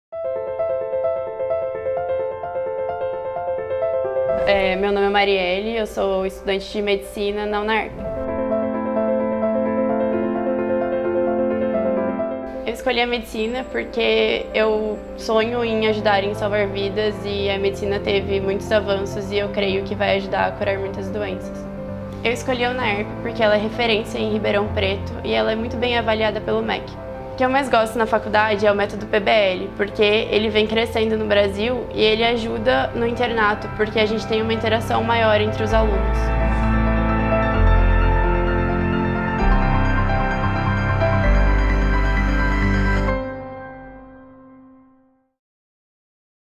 Venha Conhecer nosso Curso - Unaerp
Confira no vídeo uma dessas histórias, a de Mariele que cursa Medicina em uma das melhores universidades particulares do Brasil, a UNAERP.